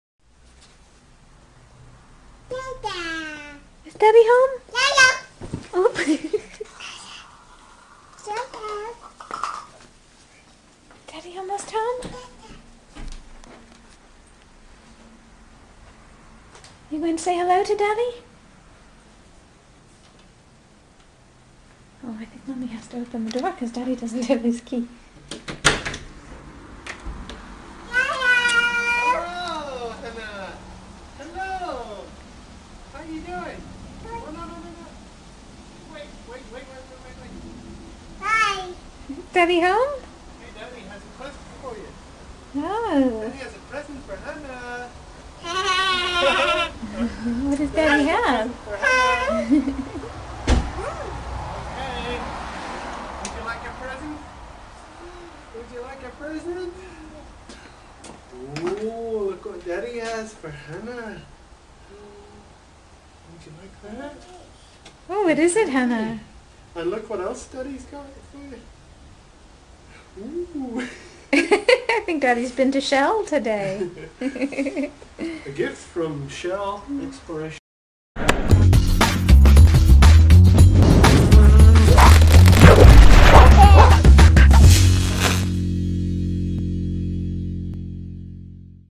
Watch this baby girl preciously greet her daddy home from work
Hannah preciously greets her dad when he comes home from work in this heartwarming clip. What a sweetheart!